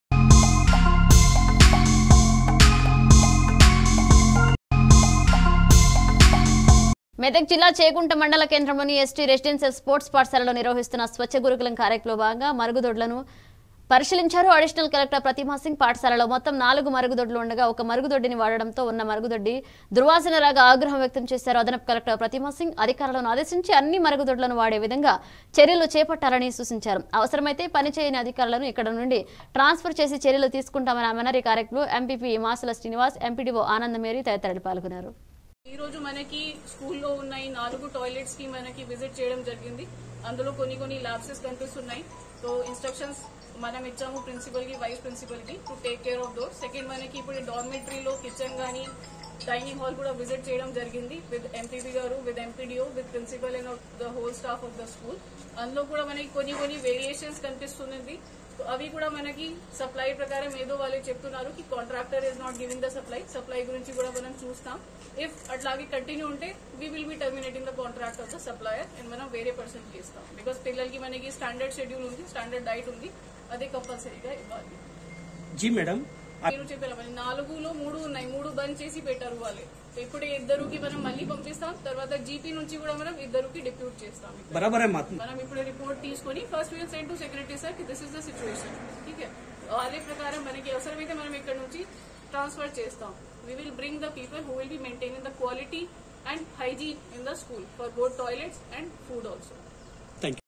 చేగుంట లోని.ఎస్టి. రెసిడెన్షియల్ స్పోర్ట్స్ పాఠశాల ను పరిశీలించిన అడిషనల్ కలెక్టర్ ప్రతిమ సింగ్
చెగుంట మండల కేంద్రం లోని.ఎస్టి. రెసిడెన్షియల్ స్పోర్ట్స్  పాఠశాల లో నిర్వహిస్తున్న  స్వచ గురుకులం కార్యక్రమoలో భాగంగా మరుగు దొడ్లను  పరిశీలించిన అడిషనల్ కలెక్టర్ ప్రతిమ సింగ్